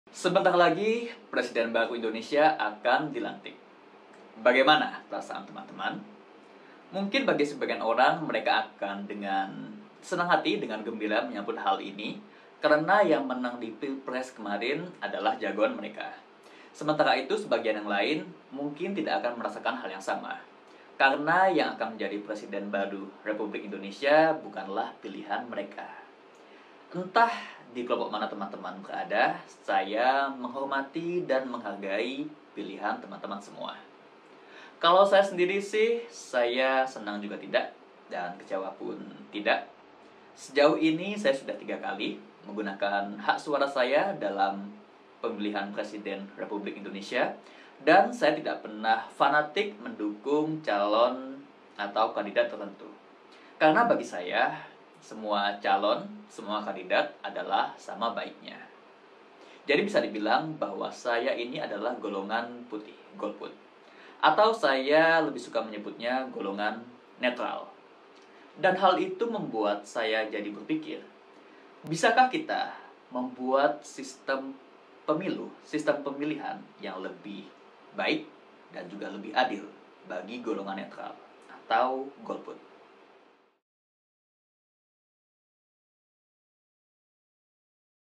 Adil bagi Golput: Mendesain Ulang Sistem Pemilihan (Part 1: Intro)
Sebentar lagi Indonesia punya presiden baru. Bagaimana pengalaman Teman-Teman saat pilpres kemarin? Bisa ga kita membuat sistem pemilihan yang lebih baik? Sambil menunggu part 2 video ini dirilis, silakan brainstorming ide di kolom komentar ya 😁